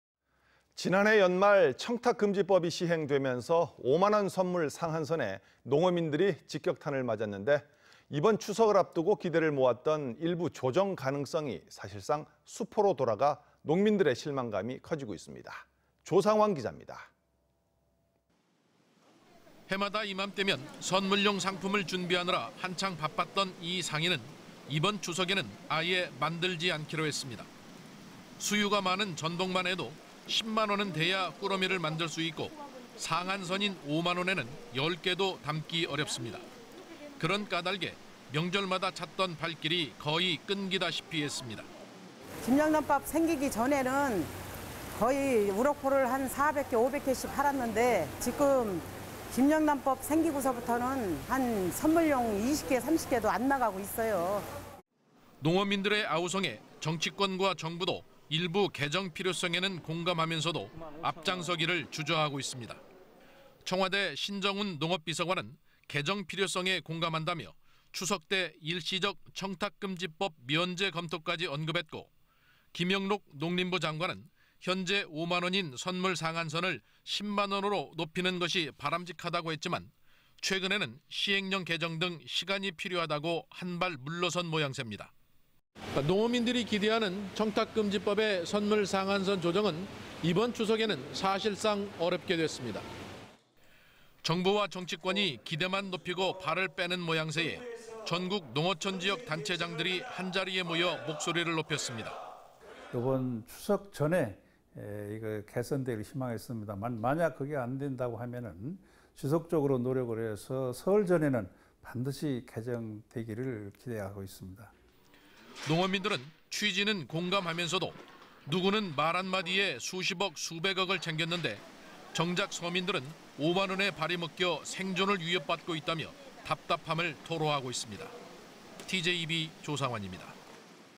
[TJB뉴스] 청탁금지법 개정 무산 농민 실망
【앵커멘트】
지난해 연말
청탁금지법이 시행되면서
5만원 선물 상한선에
농어민들이 직격탄을
맞았는데,

이번 추석을 앞두고
기대를 모았던
일부 조정 가능성이
사실상 수포로 돌아가
농민들의 실망감이

조상완 기자.